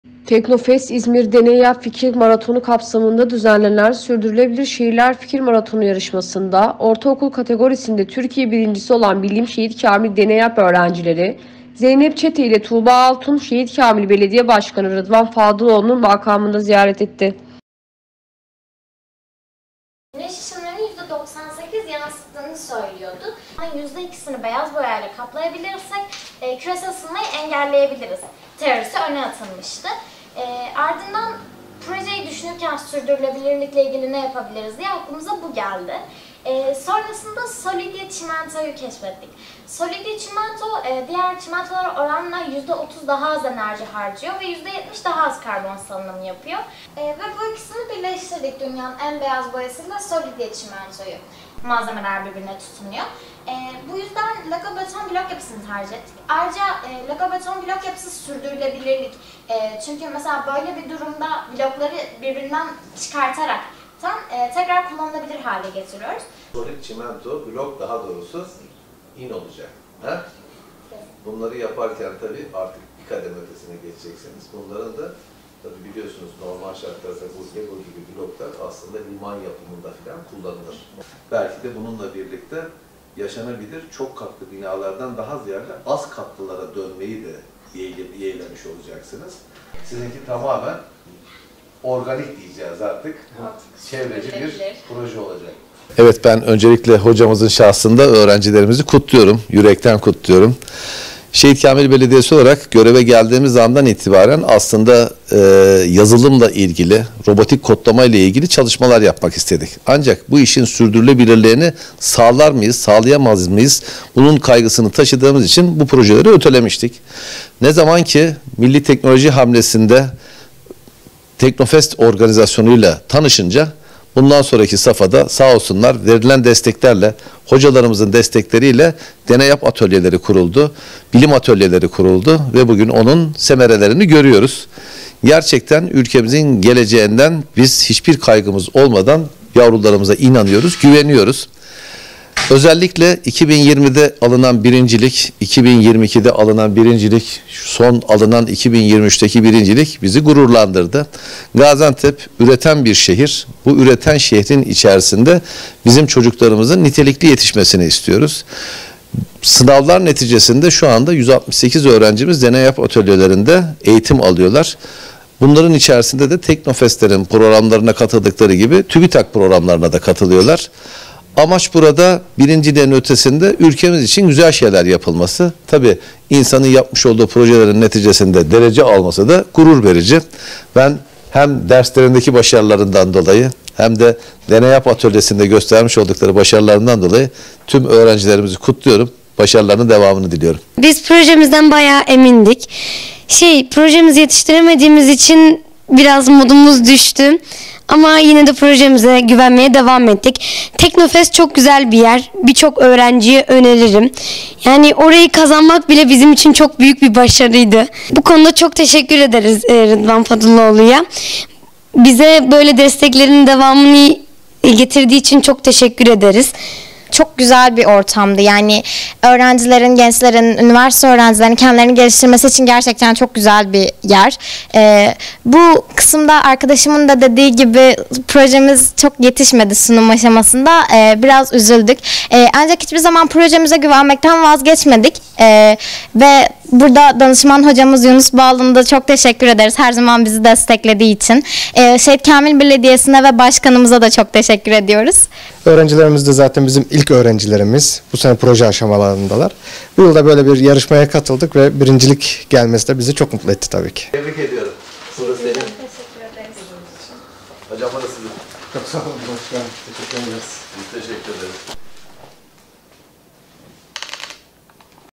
Başkan Fadıloğlu, Şampiyonları Tebrik Etti
TEKNOFEST İZMİR Deneyap Fikir Maratonu kapsamında düzenlenen Sürdürülebilir Şehirler Fikir Maratonu Yarışması’nda ortaokul kategorisinde Türkiye 1.’si olan Bilim Şehitkamil Deneyap öğrencileri Zeynep Çete ile Tuğba Altun, Şehitkamil Belediye Başkanı Rıdvan Fadıloğlu’nu makamında ziyaret etti.